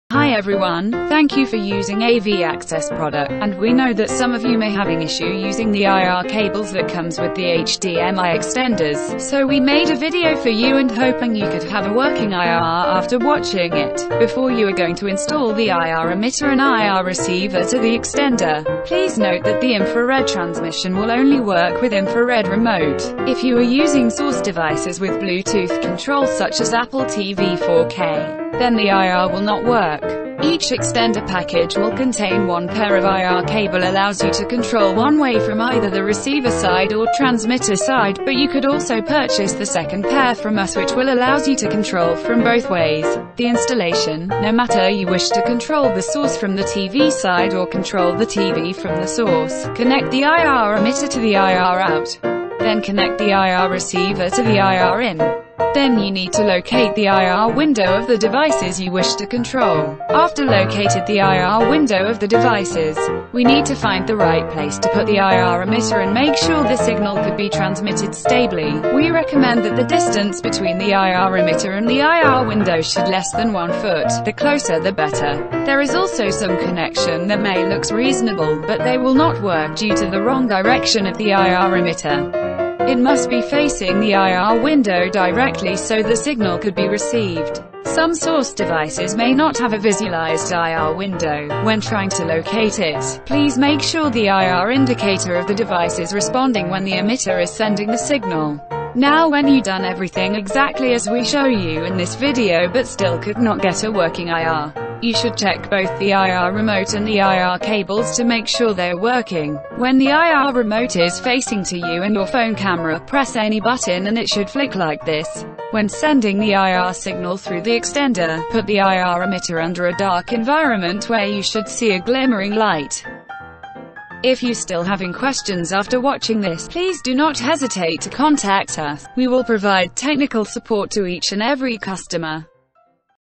IR trouble shooting
If you want to know how to set up the IR extension cables that comes within the box or having issue using them. This is the video for you.